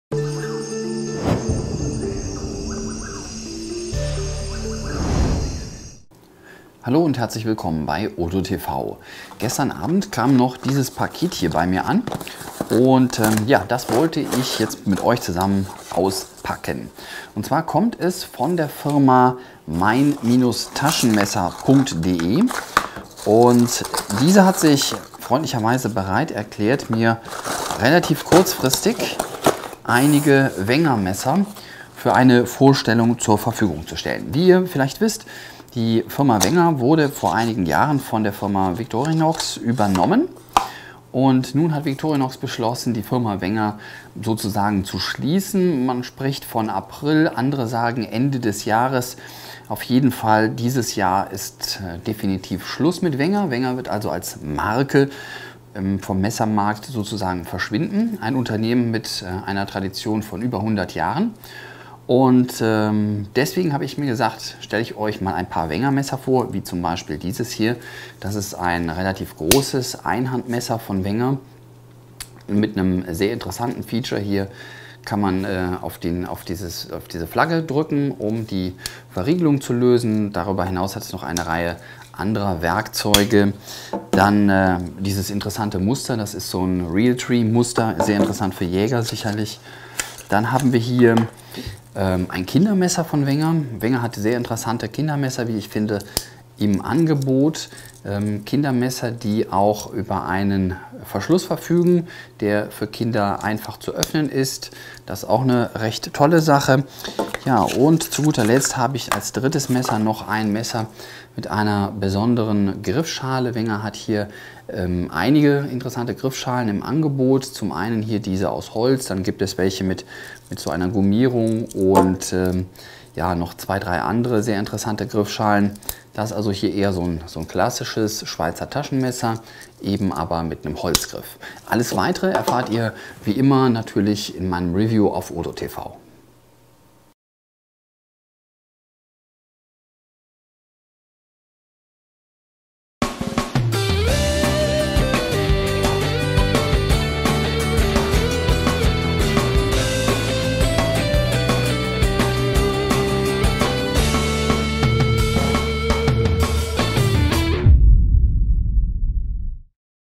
Unboxing Wenger Taschenmesser
Bevor Wenger dieses Jahr geschlossen wird, haben wir einige der Schweizer Taschenmesser geordert. Gestern eingetroffen sind das Wenger Ranger Hardwoods 57, das Wenger EvoWood 14 und das Wenger EvoGrop 10 Kindermesser.